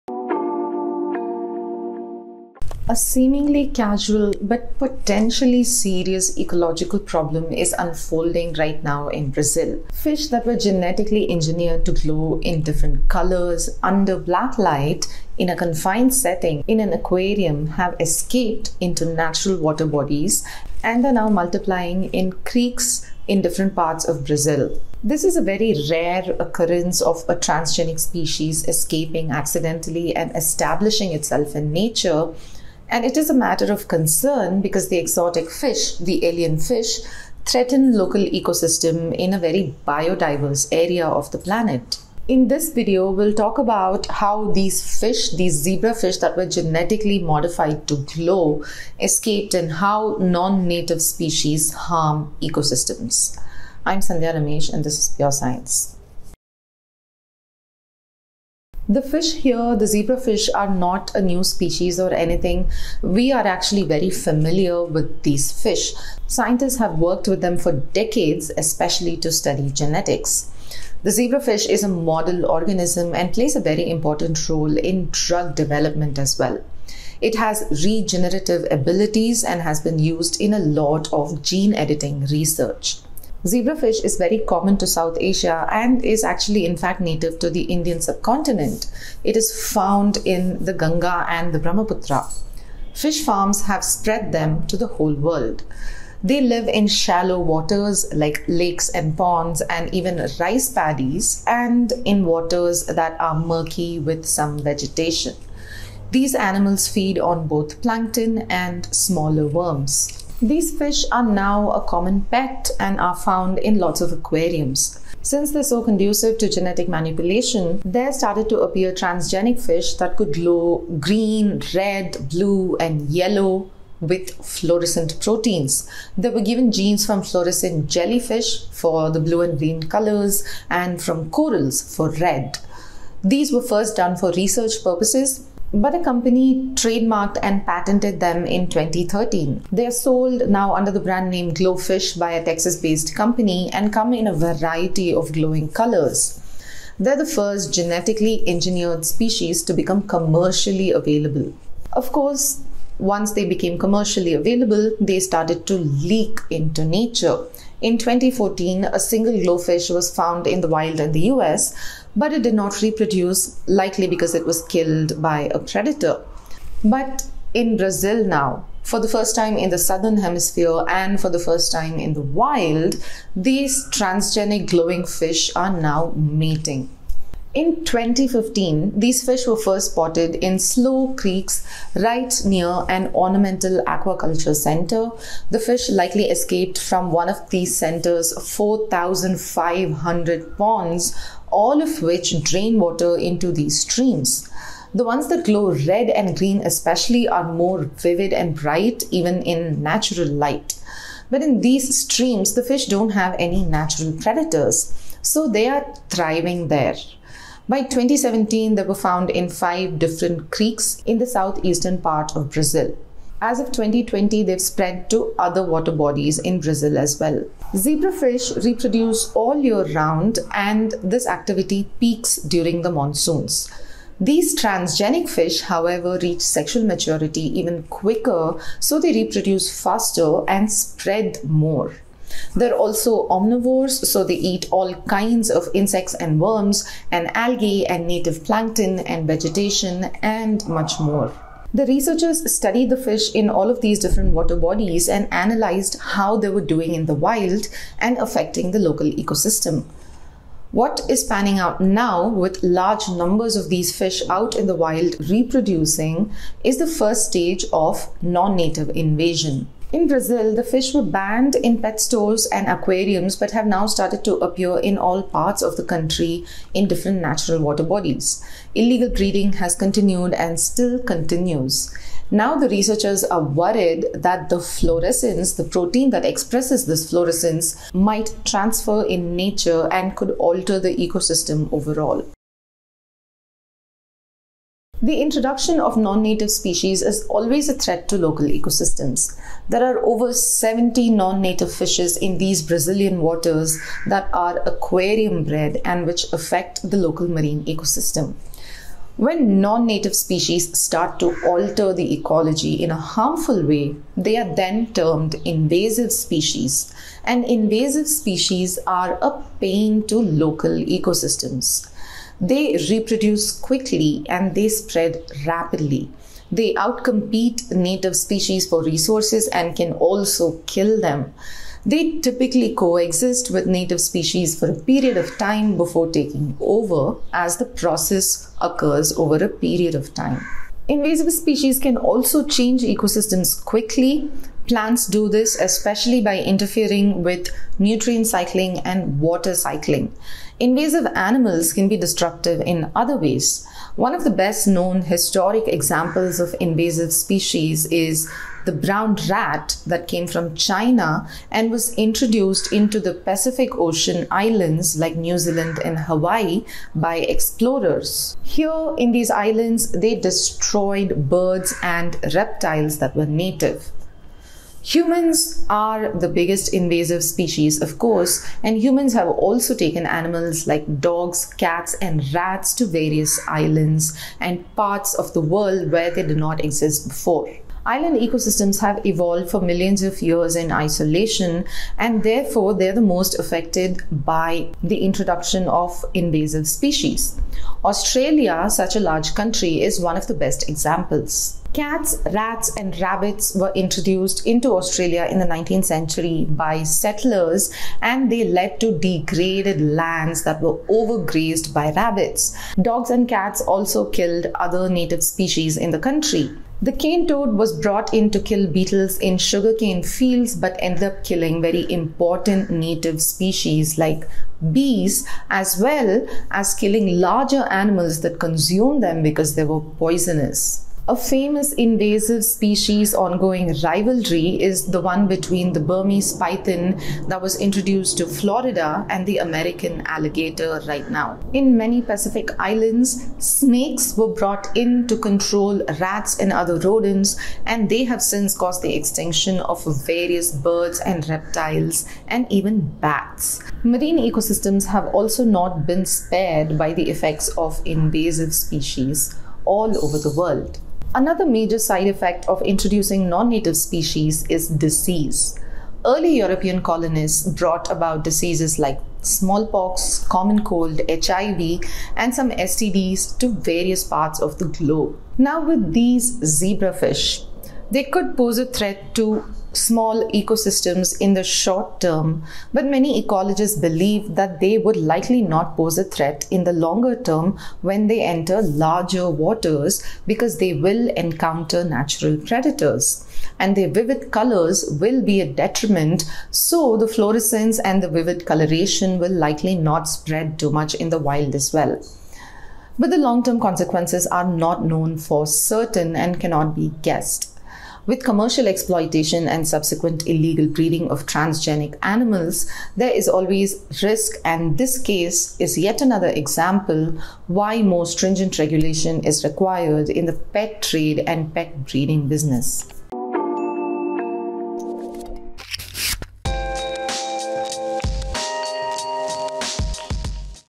Transgenic glowing aquarium fish escape in Brazil
Transgenic zebrafish engineered to glow various colors have escaped likely illegal aquariums in Brazil into natural waters. The fish are now observed to be mating in the wild and growing in numbers. ThePrint’s Sandhya Ramesh explains more.

Supplementary reading: Magalhães, et al., 2022, The fluorescent introduction has begun in the southern hemisphere: presence and life-history strategies of the transgenic zebrafish Danio rerio (Cypriniformes: Danionidae) in Brazil, Studies on Neotropical Fauna and Environment,